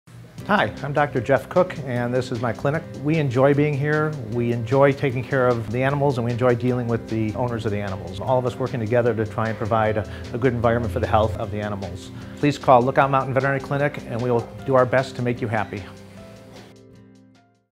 Lookout Mountain Veterinary Clinic - Short | Phoenix, AZ
At Lookout Mountain Veterinary Clinic, we specialize in veterinary care, dentistry, and surgery. In addition our facility is equipped with x-rays, nutrition counseling, and behavior counseling. We are located in Phoenix and serve the surrounding communities of Glendale and Scottsdale.